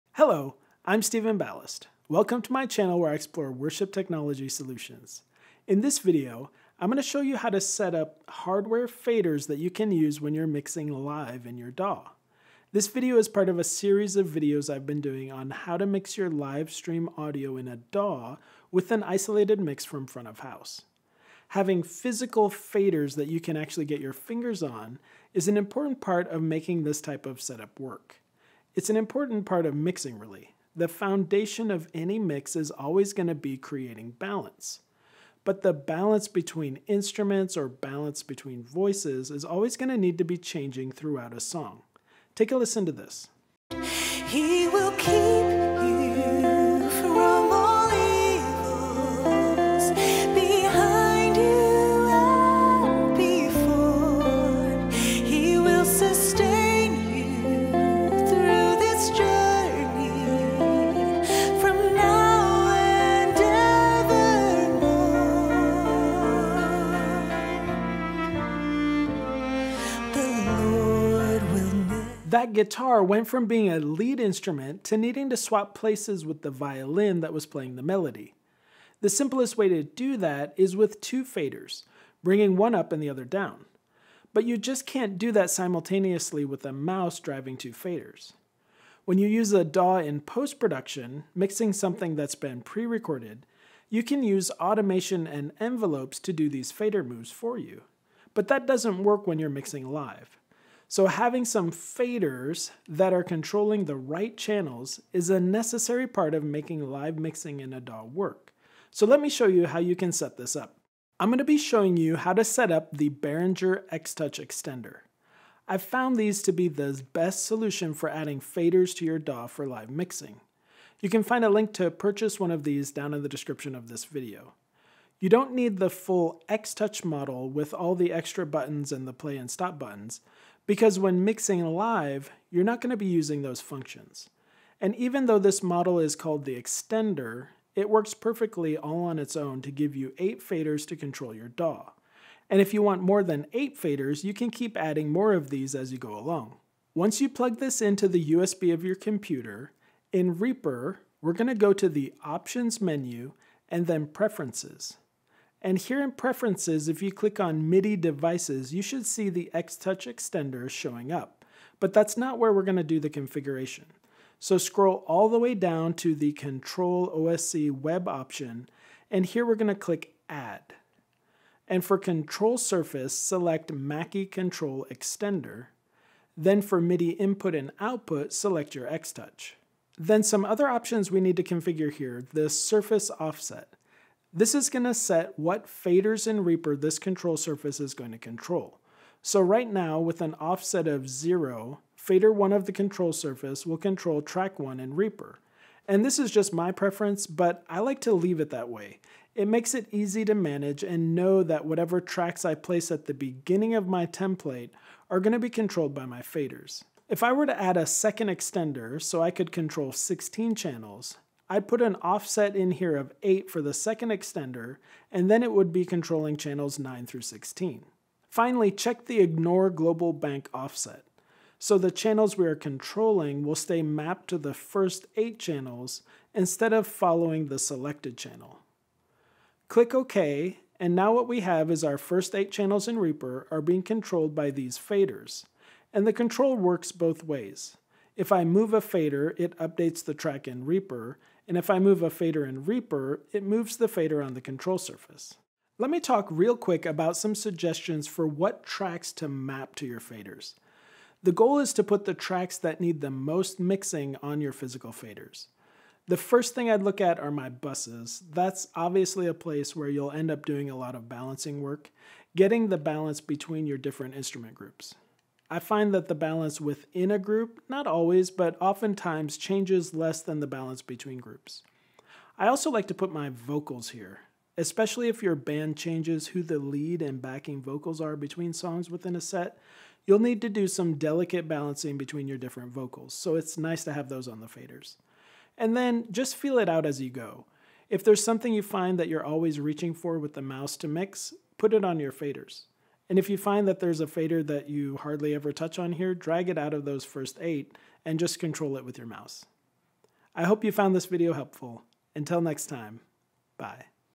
Make My Live Stream Sound Better - Mixing in a DAW Live with Hardware Faders
In this video I'll show you how to connect hardware faders to your DAW for live mixing.  This video is part of a series of videos on how to make your live streams sound better!

X-TOUCH EXTENDER